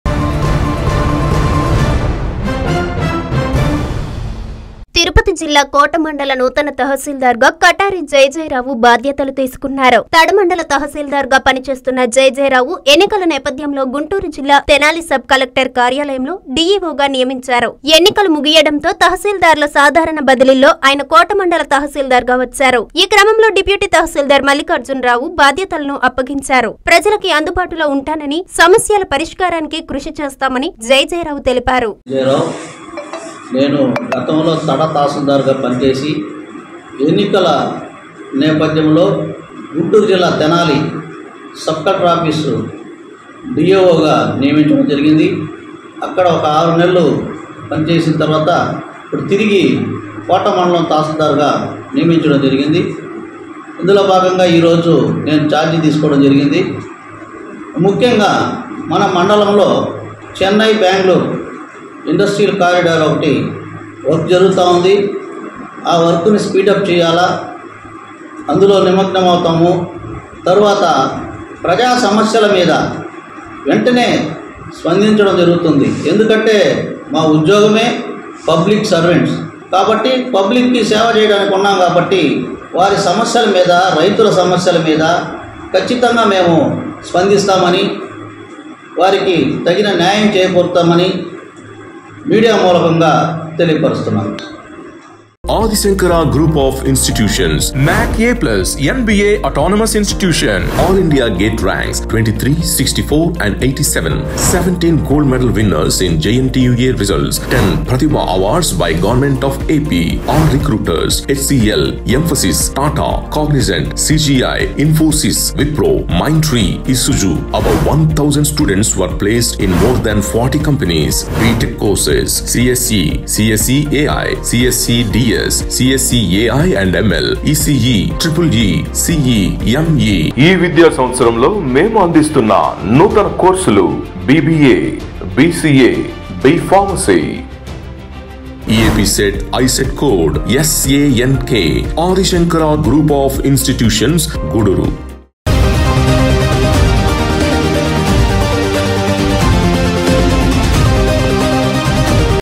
ప్ర‌జ‌ల‌కి అందుబాటులో ఉంటా
- కోట నూత‌న త‌హ‌సీల్దార్‌గా బాధ్య‌తలు స్వీక‌రించిన క‌టారి జ‌య‌జ‌య‌రావు